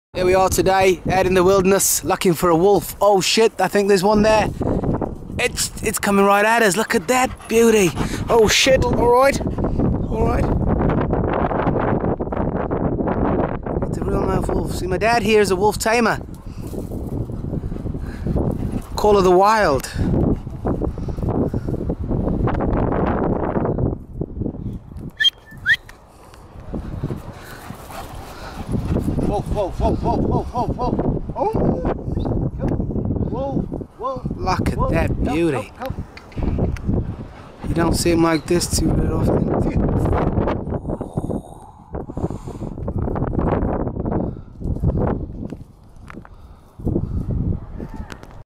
wild wolf
wolf and shepard having a walk